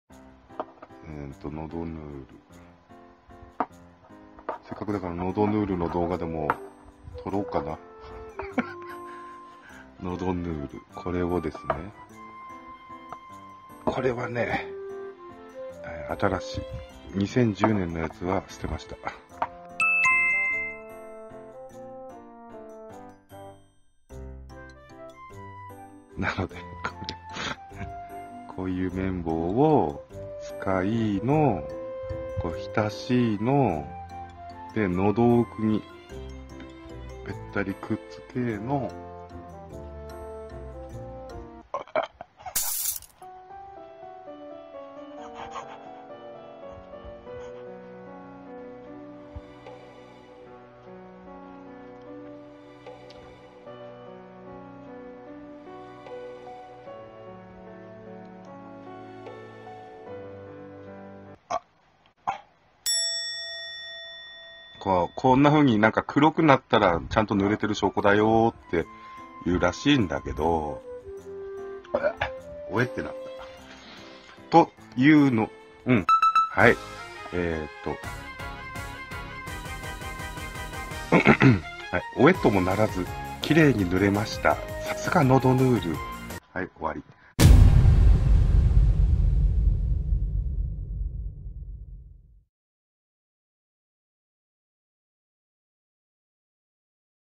心霊なの？喉痛なの？のどぬ～るをぬるだけの動画
喉が腫れていたくなったので
のどぬ～るを塗るだけの動画を撮りましたさ☆
ただそれだけｗ